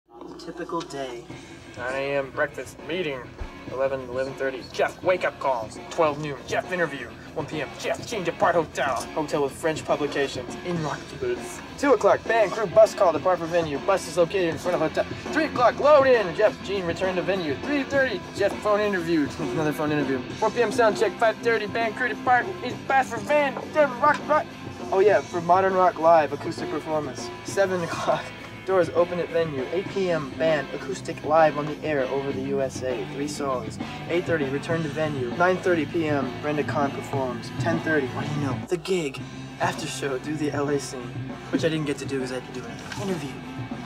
A Day in the Life of Jeff Buckley
SME

Video montage of Jeff Buckley's daily itinerary, while on tour.